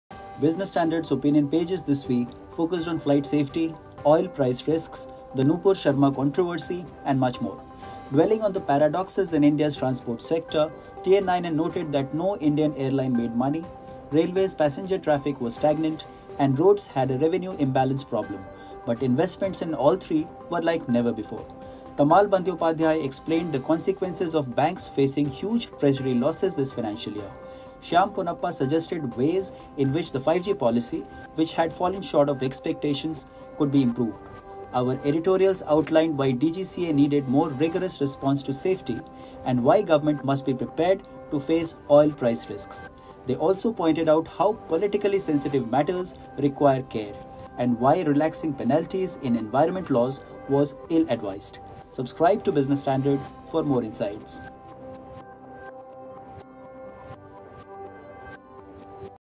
BS Views of the week: Flight safety, oil price risks, 5G policy, and more
Business Standard's opinion pages this week focused on flight safety, oil price risks, SC-Nupur Sharma controversy, and much more